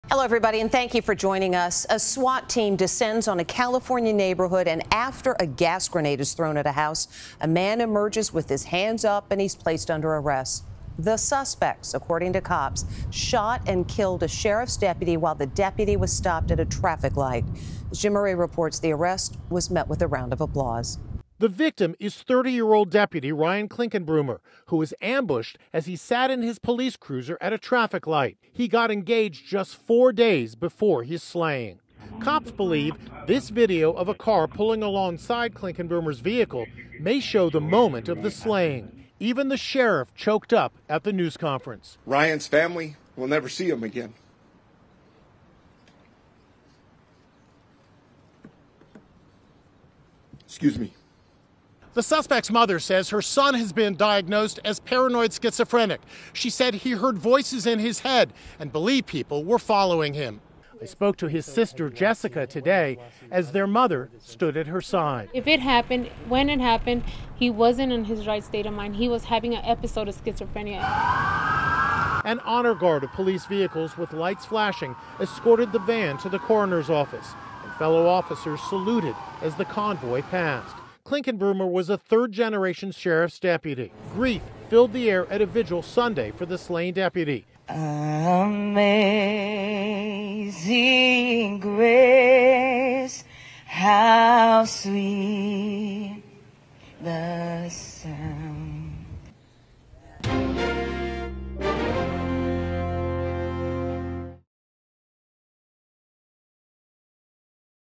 Los Angeles County Deputy Shot Dead in Ambush at Red Light
Grief filled the air at a vigil for a slain Los Angeles deputy. The victim is Deputy Ryan Clinkunbroomer, who was ambushed as he sat in his police cruiser at a traffic light. According to police, the suspect shot and killed the sheriff's deputy while he was stopped at a traffic light. Cops say the suspect barricaded himself in a house for hours. He finally came out with his hands in the air in the pre-dawn operation.